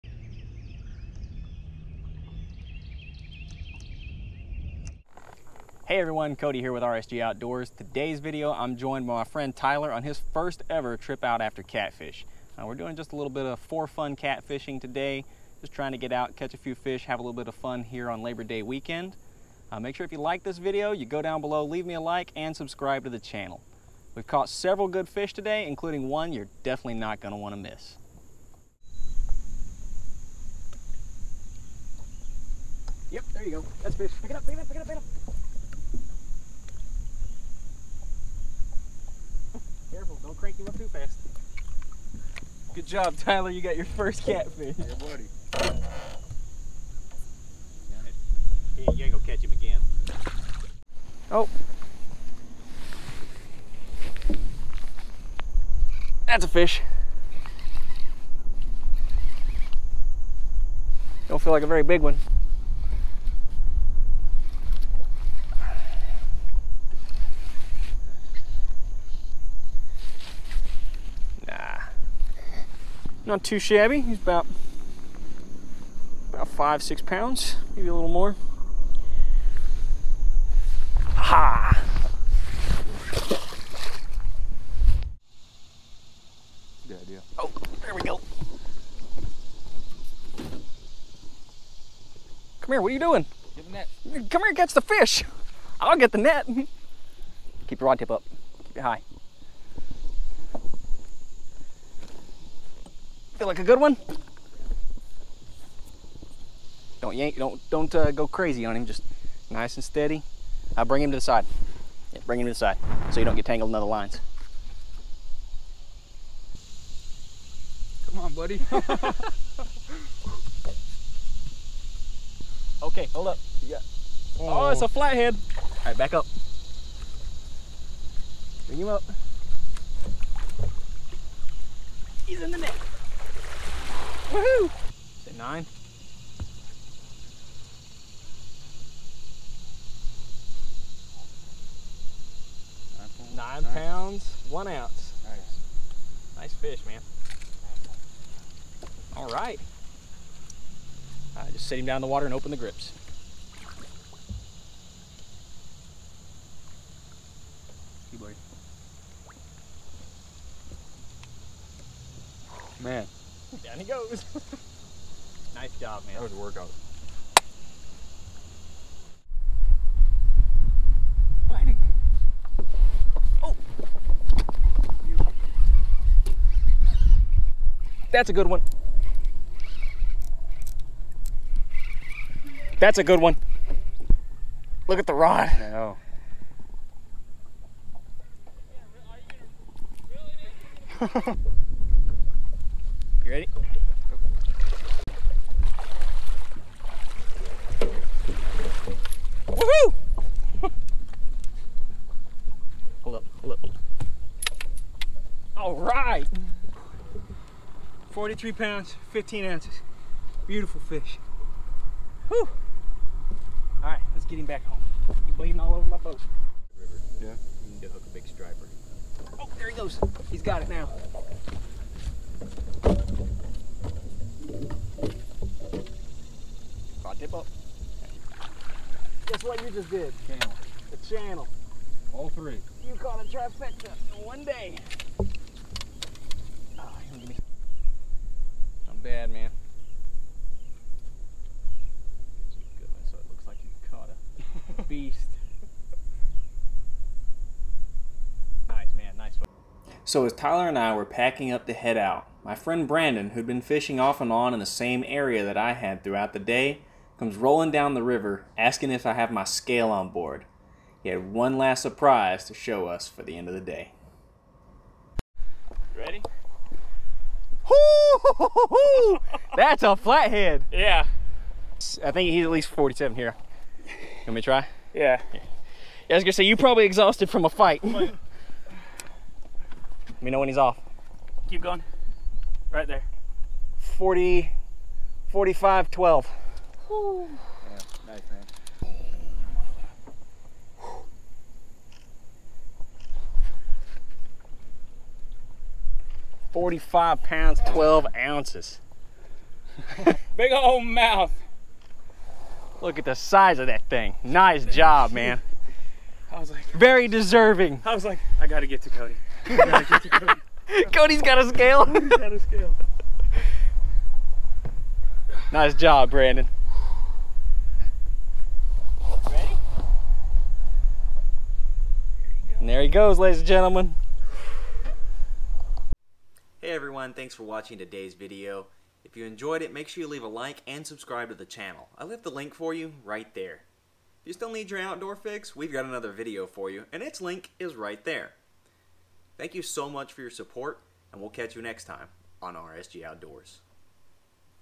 1st Time Catfishing!
So for Labor Day weekend, I took my friend Tyler to catch some catfish. He had never caught one before, and we managed to get him a trifecta! All three major species of catfish in one day. He also got to see a couple nice sized fish too. Enjoy!